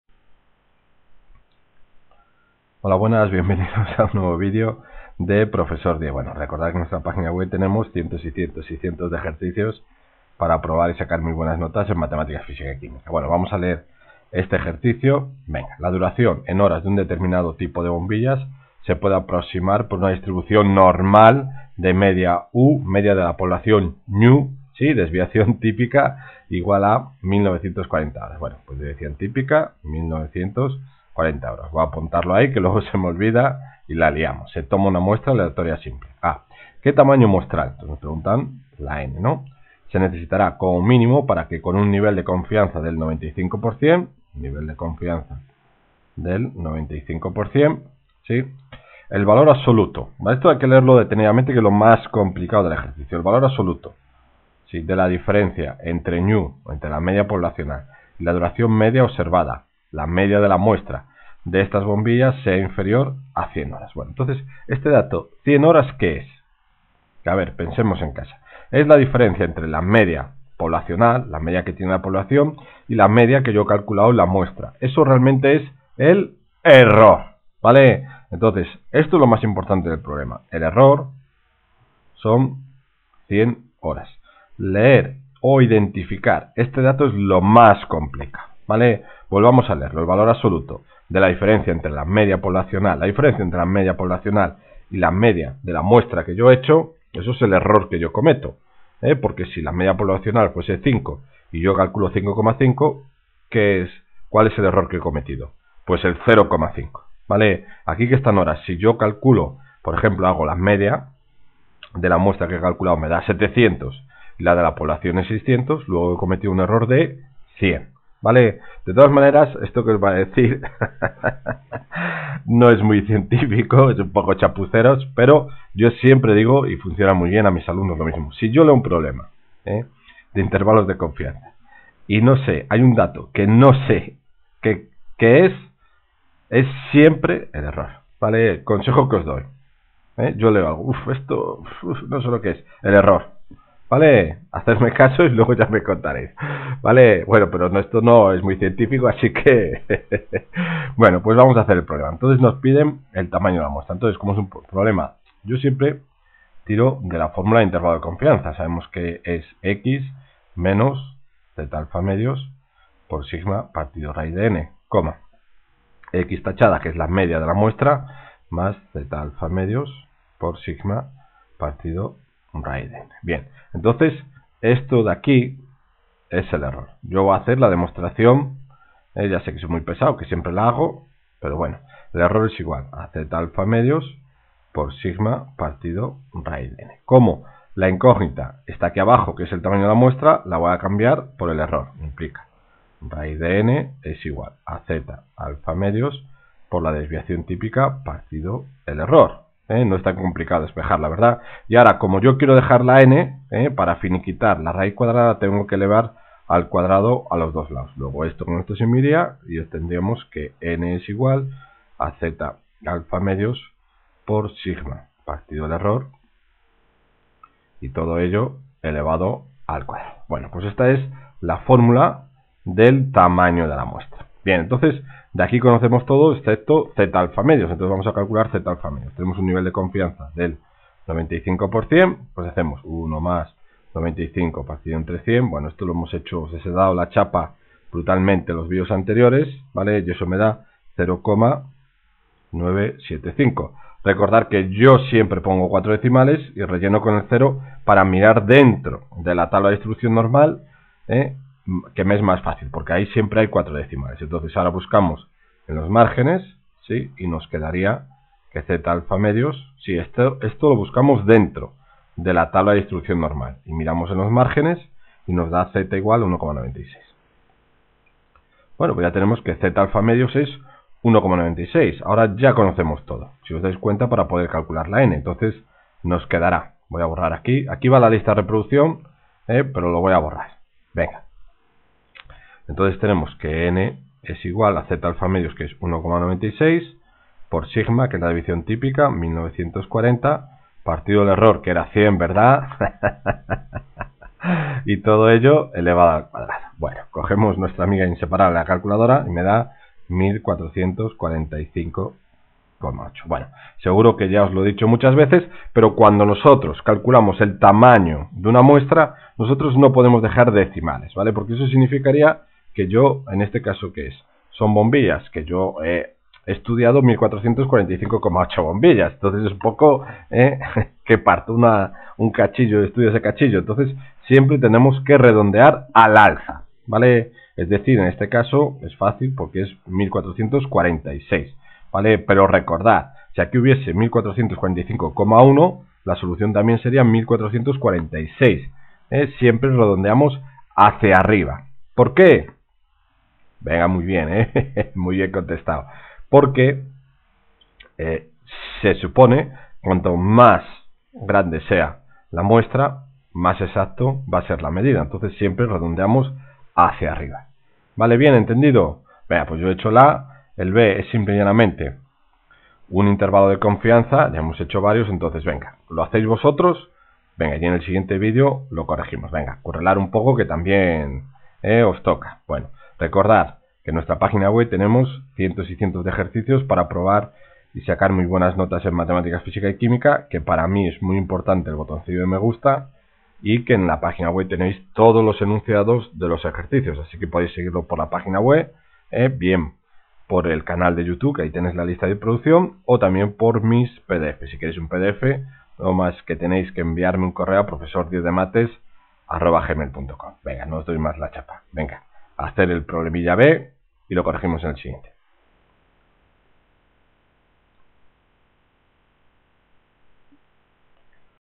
Intervalo de confianza 09 para la media ejercicio resuelto
Selectividad PAU matemáticas aplicadas a las ciencias sociales Madrid Junio 2013 5 B
La duración en horas de un determinado tipo de bombillas se puede aproximar por una distribución normal de media µ y desviación típica igual a 1940 h. Se toma una muestra aleatoria simple.
a) Qué tamaño muestral se necesitará como mínimo para que, con nivel
de confianza del 95 %, el valor absoluto de la diferencia entre µ y la
duración media observada de esas bombillas sea inferior a 100 h?
b) Si el tamaño de la muestra es 225 y la duración media observada X es
de 12415 h, obténgase un intervalo de confianza al 90% para µ